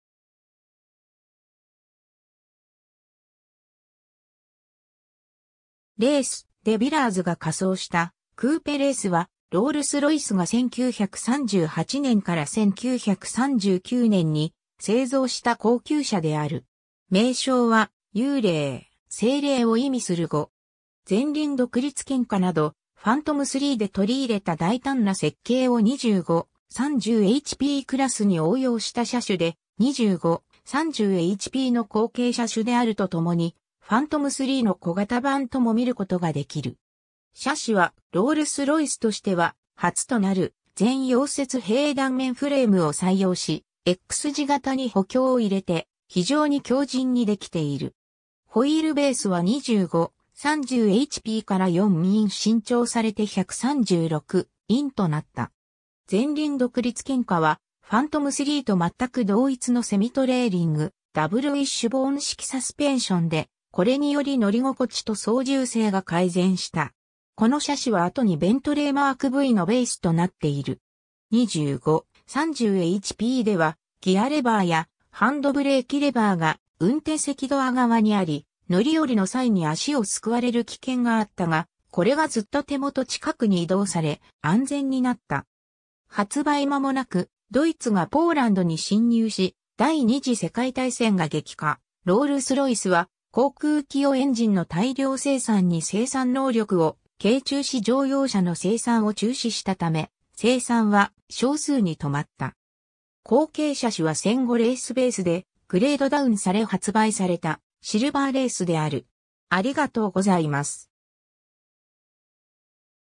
ロールス・ロイス・レイス
ロールス・ロイスの車種
レイス、De Villarsが架装したクーペ レイス（Wraith ）はロールス・ロイスが1938年から1939年に製造した高級車である。
名称は「幽霊」「生霊」を意味する語（スコットランドの語）。
 前輪独立懸架などファントムIIIで取り入れた大胆な設計を25/30HPクラスに応用した車種で、「25/30HPの後継車種」であるとともに「ファントムIIIの小型版」とも見ることができる。
 シャシはロールス・ロイスとしては初となる全溶接閉断面フレームを採用し、X字型に補強を入れて非常に強靭にできている。
ホイールベースは25/30HPから4in伸長されて136inとなった。
前輪独立懸架はファントムIIIと全く同一のセミトレーリング・ダブルウィッシュボーン式サスペンションで、これにより乗り心地と操縦性が改善した。
このシャシは後にベントレー・マークVのベースとなっている。
 25/30HPではギアレバーやハンドブレーキレバーが運転席ドア側にあり、乗り降りの際に足を掬われる危険があったが、これがずっと手元近くに移動され、安全になった。
 発売間もなくドイツがポーランドに侵入し第二次世界大戦が激化、ロールス・ロイスは航空機用エンジンの大量生産に生産能力を傾注し乗用車の生産を中止したため、生産は少数に留まった。
後継車種は戦後レイスベースでグレードダウンされ発売されたシルヴァーレイスである。